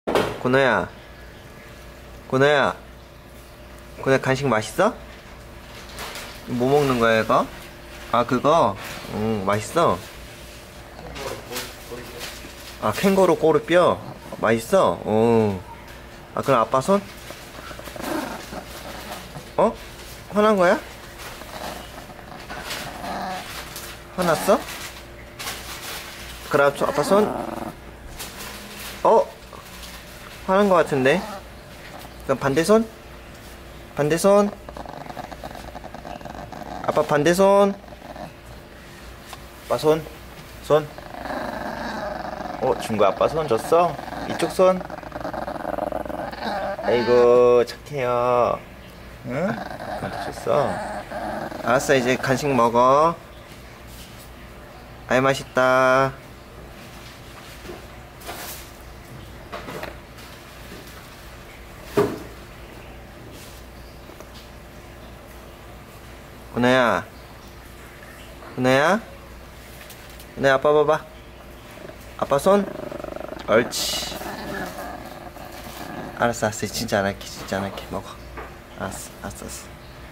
캥거루꼬리뼈 먹는 강아지에게 "손" 해보기ㅋㅋㅋ[정브르]
고나에게 간식을 줬어요~!! 수제간식이라 넘나 좋아하는 뼈간식인데~~!! 먹다가 문득 여러분들에게 고나가 얼마나 착한지 보여주고 싶어서 영상을 찍엇어요~~

싫을만도 한데..큰아빠랑 작은아빠의 장난을 받아주는 우리 착한고나~~~